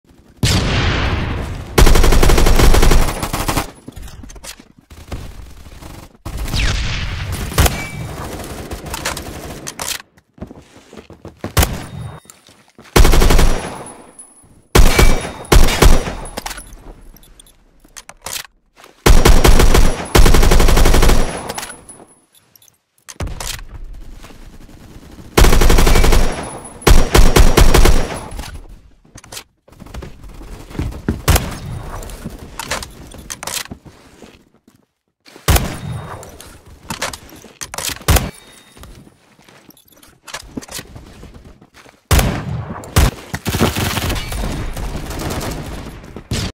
POV: You spawn camp with a battle rifle
The fal 50 is reasonable and the saiga 12u is a totally also reasonable secondary shotgun that you can put an extended mag on, so balanced.